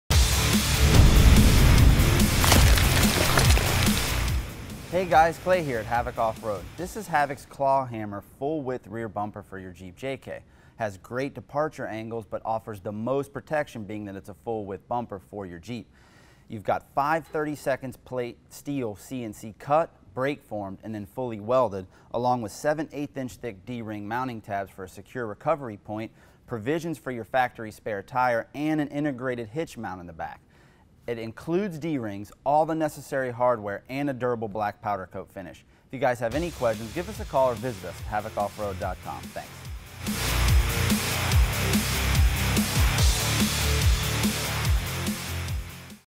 Havoc Clawhammer Rear Bumper Review - HavocOffroad.com
Klay reviews the Havoc Clawhammer Rear Bumper.